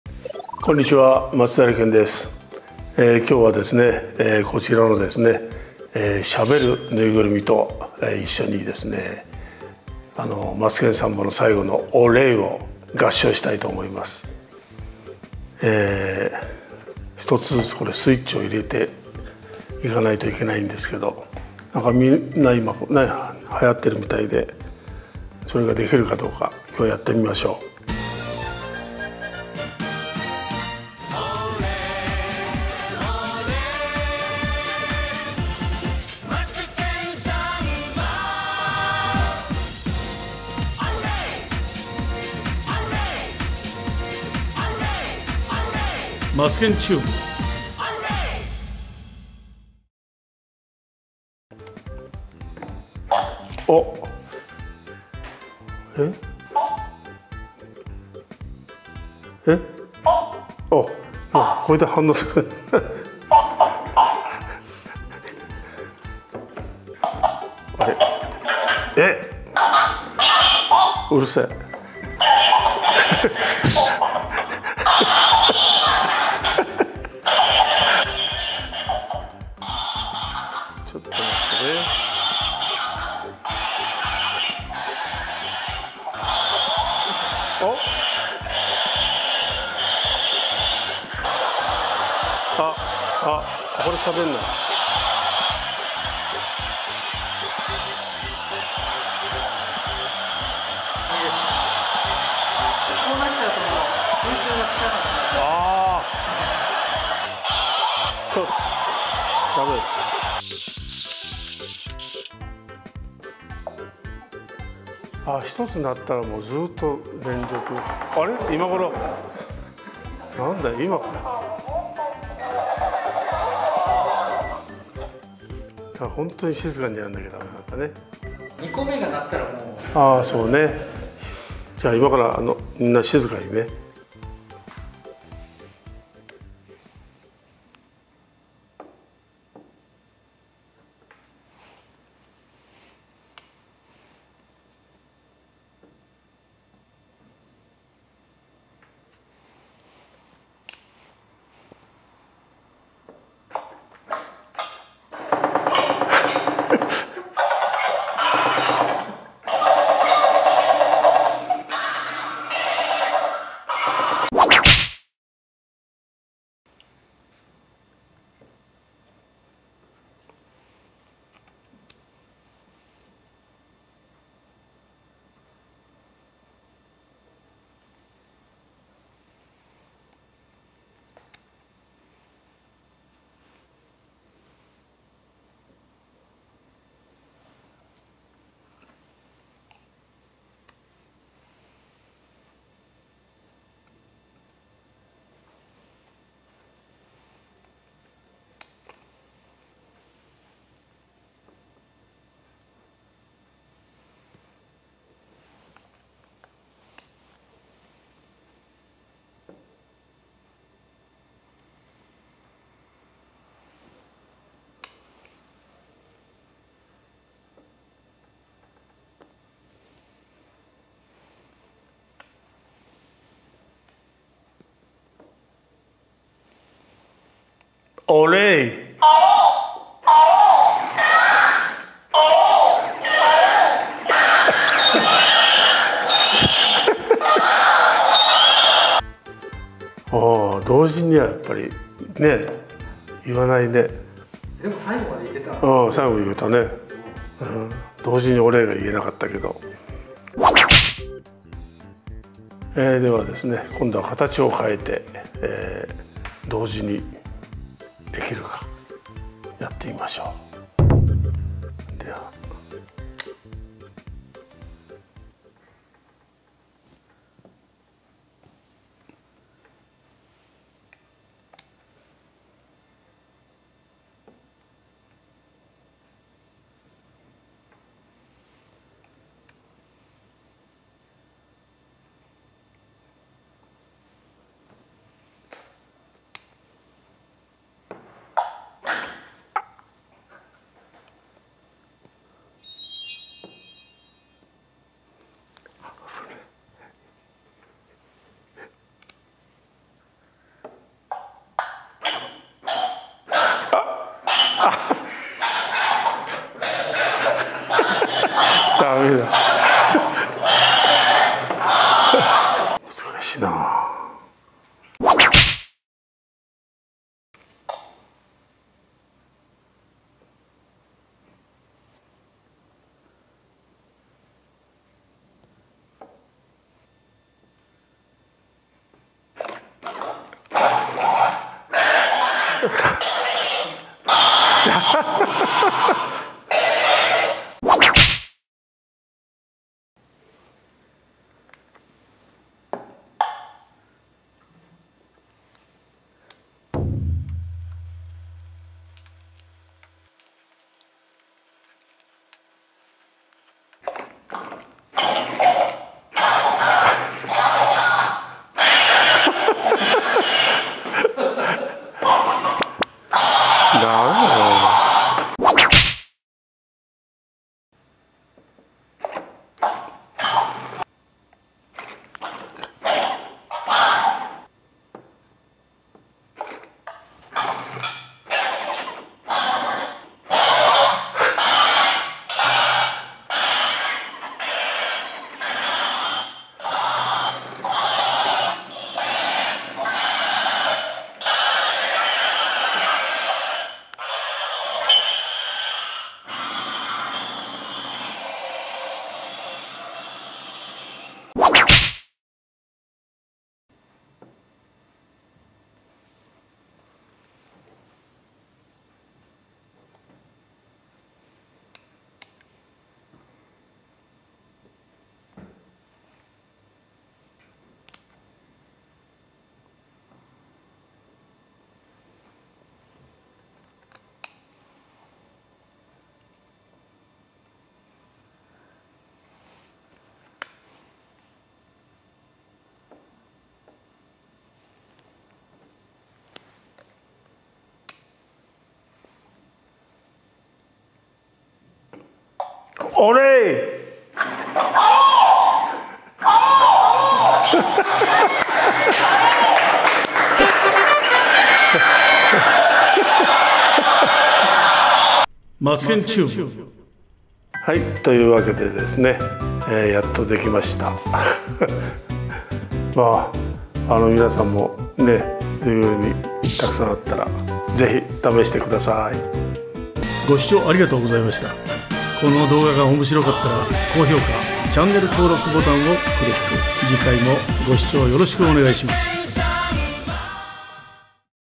マツケンvsぬいぐるみ
アンジャッシュ大島さんがやっていたのをやってみました。

また、各種問合せ先はチャンネル管理アドレス

までご連絡をお願い致します。

（マツケンTube Vol.89）